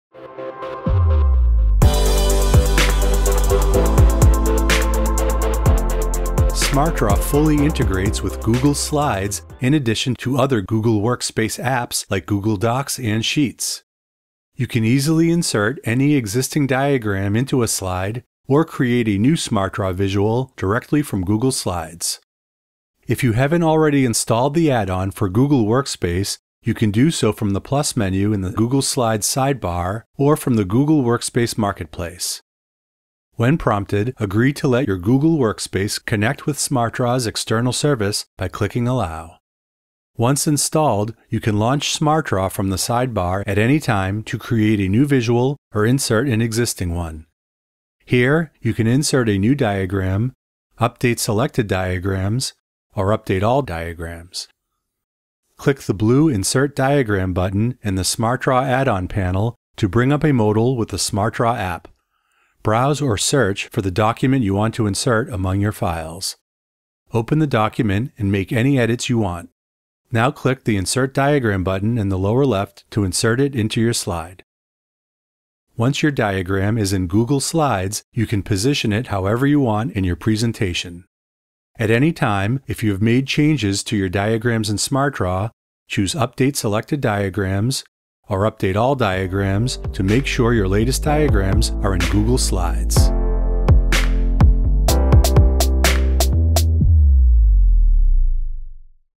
Add Diagrams to Google Slides
SmartDraw now fully integrates with Google Slides in addition to other Google Workspace™ apps like Google Docs™ and Sheets™. You can easily insert any existing diagrams into a slide or create a new SmartDraw visual directly from Google Slides™.